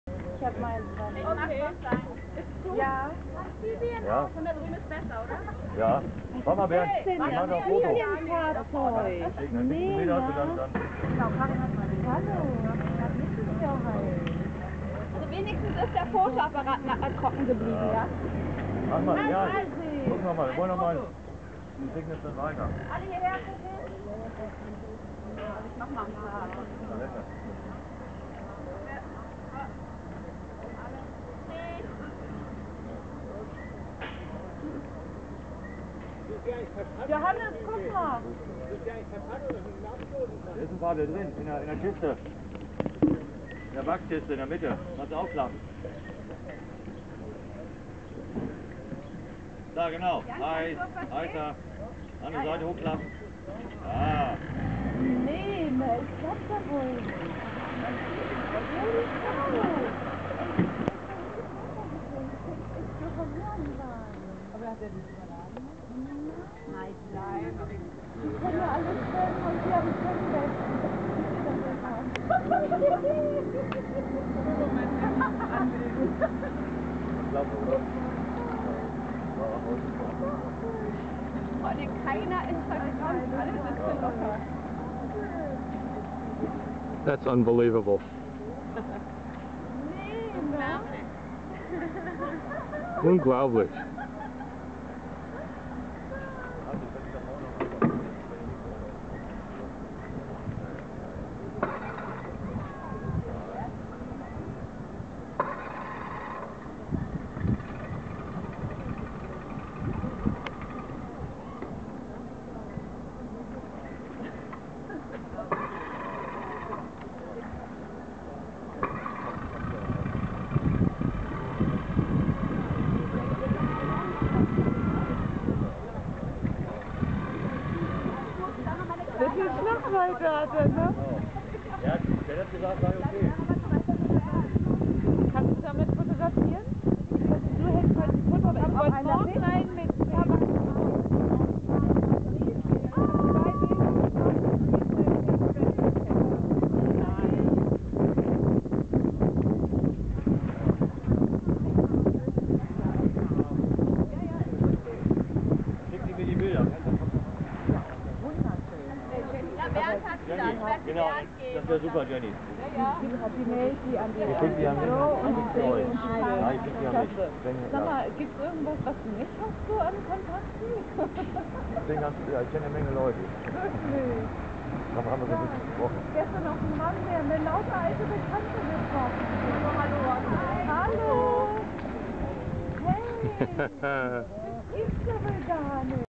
Amphicar Launch in Berlin
One of the 1000 or so remaining original Amphicars, launched by a guy we ran into in Berlin, who invited us to the Wansee marina to see the launch. He invited all the little kids for a quick spin, and here it is.